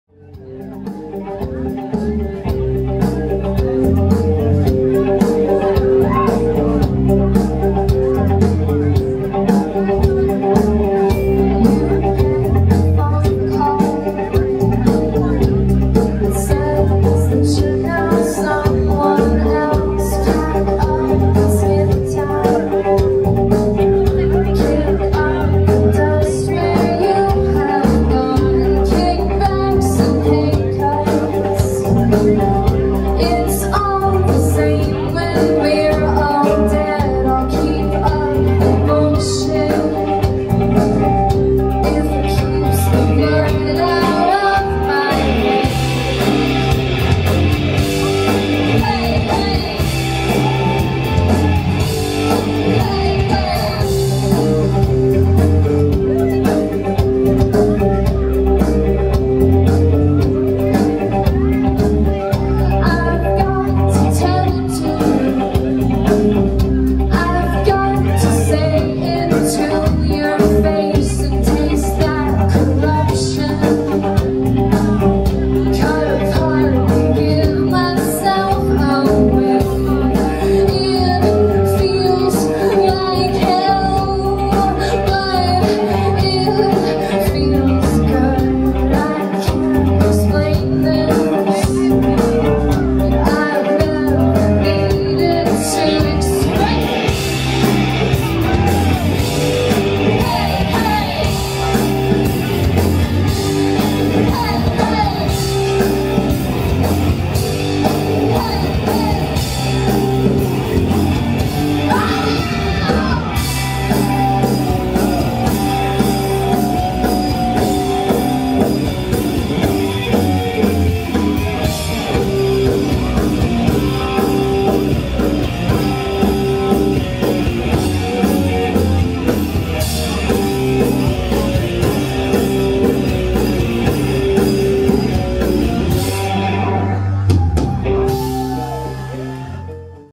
The White White Lights at Stubb's Austin, TX 1.5.2012
The White White Lights inside at Subb's in Austin, Texas -1.5.2012
Free Week in many Austin, TX clubs.

Panasonic GH2
20mm f/1.7
44mb Hack